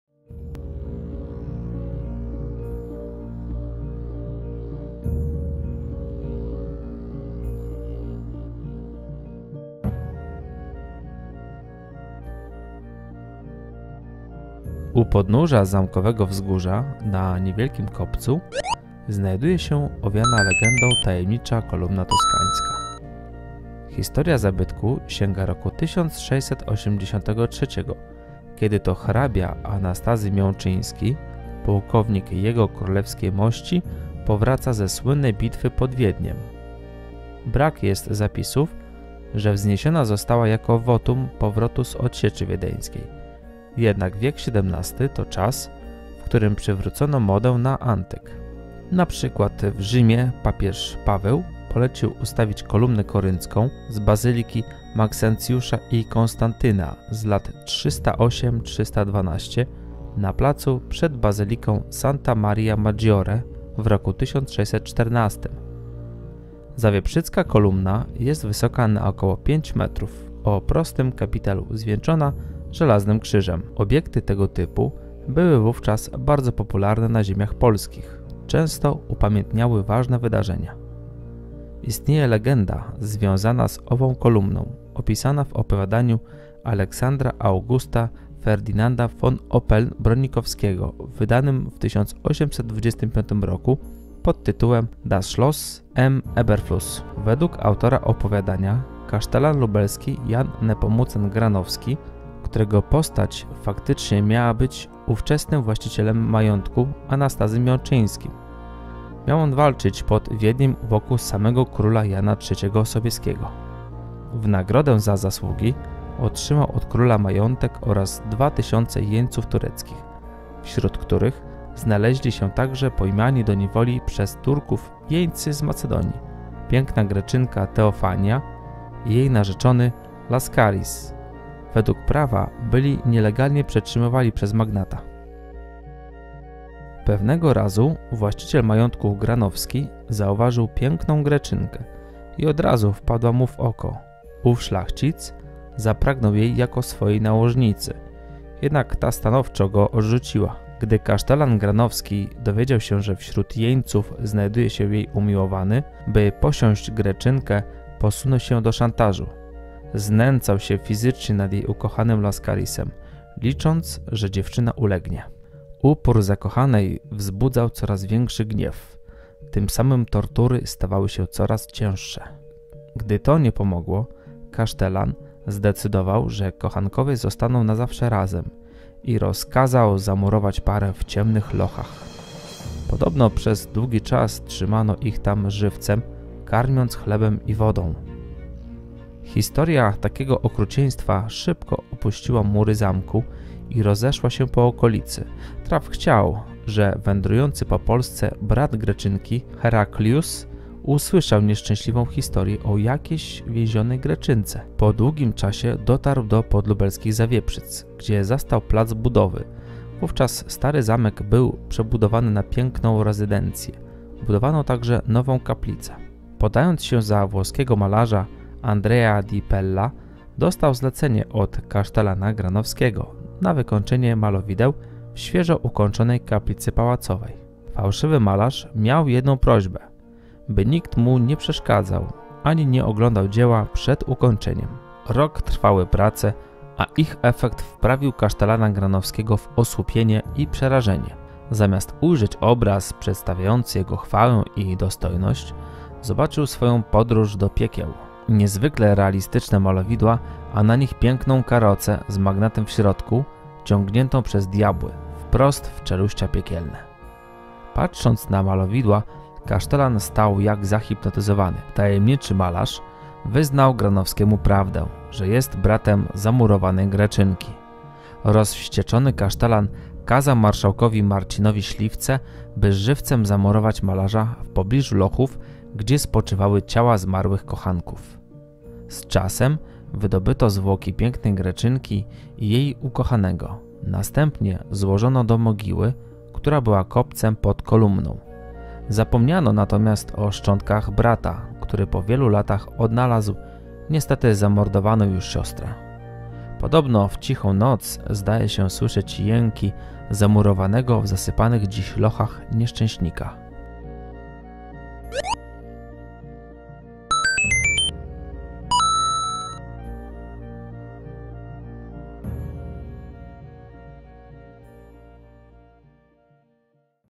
Kolumna Toskańska w Zawieprzycach i legenda o Zamku
U podnóża zamkowego wzgórza na niewielkim kopcu znajduje się owiana legendą  tajemnicza kolumna Toskańska.

Historia zabytku sięga roku 1683 kiedy to hrabia Anastazy Miączyński pułkownik Jego Królewskiej Mości  powraca ze słynnej bitwy pod Wiedniem.
Brak jest zapisków, że wzniesiona została jako wotum  powrotu z odsieczy wiedeńskiej jednak wiek XVII to czas, w którym przywrócono modę na antyk np. w  Rzymie papież Paweł polecił ustawić  kolumnę koryncką z bazyliki Maksencjusza i Konstantyna z lat 308 – 312  na placu przed bazyliką Santa Maria Maggiore w roku 1614.
Zawieprzycka kolumna jest wysoka na około 5 metrów  o prostym kapitelu zwieńczona żelaznym. Obiekty tego typu były wówczas bardzo popularne na ziemiach polskich często upamiętniały  ważne wydarzenia .

Istnieje legenda związana z ową kolumną opisana w opowiadaniu j Alexandra Augusta Ferdinanda Von Oppeln- Bronikowskiego wydanym w 1825 pod tytułem Das Schloss am Eberflus .
Według autora opowiadania kasztelan lubelski Jan Nepomucen Granowski ( którego postać faktycznie miała być ówczesnym właścicielem majątku Anastazym Miączyńskim ) miał walczyć pod Wiedniem u boku samego króla Jana III Sobieskiego.
W nagrodę za zasługi otrzymał od króla majątek oraz 2000 jeńców tureckich wśród , których znaleźli się także pojmani do niewoli przez turków jeńcy z Macedonii , piękna Greczynka Teofania i jej narzeczony Laskaris według prawa byli nielegalnie przetrzymywani przez magnata.

Pewnego razu  właściciel majątku Granowski zauważył piękną greczynkę i od razu wpadła mu w oko ów szlachcic zapragnął jej jako swojej nałożnicy jednak ta stanowczo go odrzucała.  Gdy ksztelan Granowski dowiedział się, że wśród jeńców znajduje się jej umiłowany by posiąść greczynkę  posunął się do szantażu.
Znęcał się fizycznie nad jej ukochanym Laskarisem licząć , że dziewczyna ulegnie .
Upór zakochanej wzbudzał  coraz większy gniew tym samym tortury stawały się coraz cięższe.
Gdy to nie pomogło , kasztelan zdecydował się , że kochankowie zostaną na zawsze razem i   rozkazał zamurować parę w ciemnych lochach.
Podobno przez długi czas trzymano ich tam żywcem karmiąc chlebem i wodą.
Historia takiego okrucieństwa szybko opuściła mury zamku i rozeszła się po okolicy traf chciał , że wędrujący po Polsce brat greczynki  Heraklius usłyszał nieszczęśliwą historię o jakieś greczynce.
Po długim czasie dotarł do podlubelskich Zawieprzyc gdzie zastał plac budowy ( wówczas stary zamek był w przebudowie na piękną rezydencję , budowano także nową kaplicę ).
Podając się za włoskiego malarza Andrea di Pella dostał zlecenie od kasztelana Granowskiego na wykonanie malowideł w świeżo ukończonej kaplicy pałacowej.  Fałszywy malarz miał jedną prośbę by nikt mu nie przeszkadzał ani nie oglądał dzieła przed ukończeniem.

Rok trwały prace a ich efekt wprawił kasztelana Granowskiego w osłupienie i przerażenie zamiast ujrzeć obraz przedstawiający jego chwałę i dostojność zobaczył, swoją podróż  do piekieł.  Niezwykle realistyczne malowidła  a na nich piękna karoca z magnatem w środku ciągnięta przez diabły wprost w czeluścia piekielne.
Patrząc na malowidła kasztelan stał jak zahipnotyzowany, tajemniczy autor malowideł wyznał Granowskiemu prawdę , że jest bratem zamurowanej greczynki.
Rozwścieczony kasztelan kazał marszałkowi Marcinowi Śliwce by żywcem zamurować malarza w pobliżu lochów  gdzie spoczywały ciała zmarłych kochanków.

Z czasem wydobyto zwłoki pięknej greczynki i jej ukochanego następnie złożono do mogiły , która była kopcem pod kolumną . Zapomniano natomiast o szczątkach brata, który po wielu latach odnalazł niestety zamordowaną już siostrę.

Podobno w cichą noc zdaje się słyszeć jęki zamurowanego w zasypanych dziś lochach nieszczęśnika .